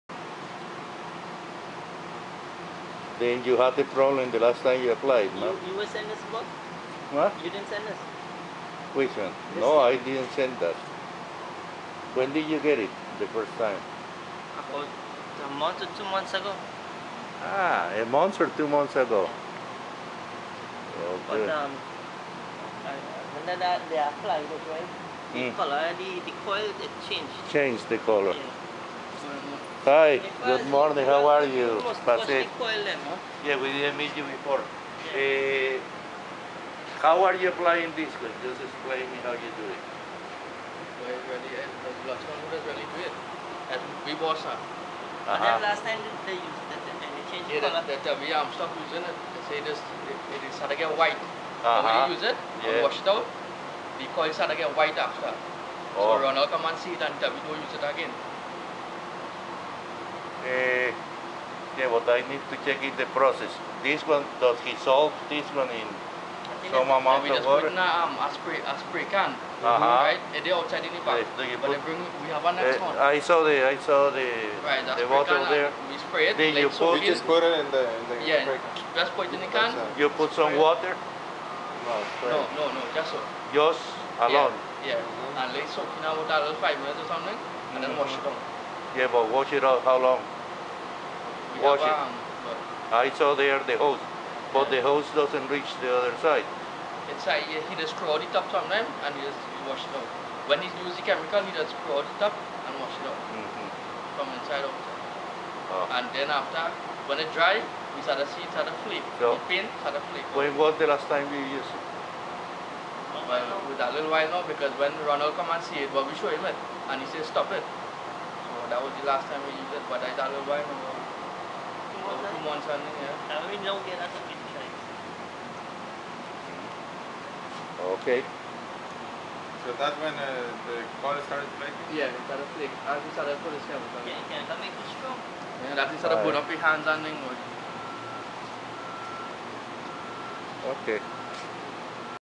Sanfernando.MP4
Interview with maintenance personnel at Pricemart Store 8004 (San Fernando) In Trinidad
They explain how Pure Coil Cleaner (Sodiun Hydroxide based) was applied and the protective coating 'changed coating color to white'
Sodiun Hydroxide is the base of common paint strippers.
in this link you will find how to build a powerful paint stripper able to remove 20 coats of lead paint.

In order to neutralize the caustic soda that has cristalyzed (see pictures atphotobucket) i would recommmend washing the coil immediately with a solution of vinegar and verify neutrality with litmus paper.

I doubt that any warranty can be honored under this conditions.